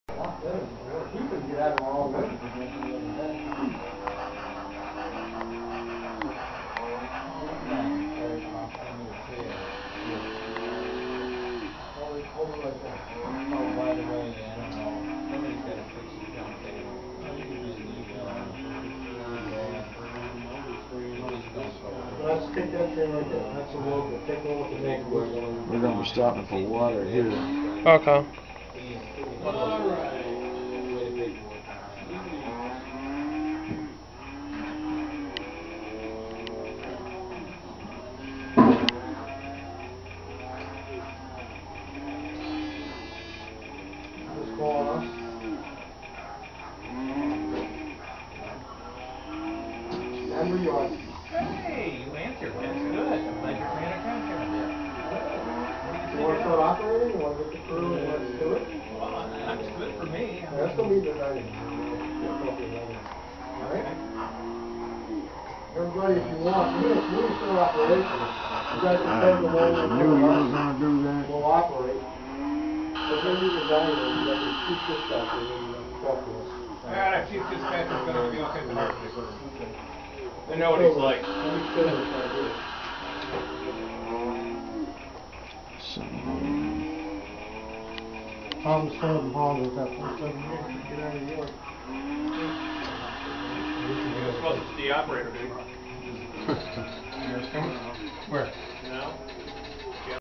Ridgway Ute & Bedrock RR Club
Took of a video of my stay at a railroad club up in Port St Lucie Florida. They have a very nice railroad club there.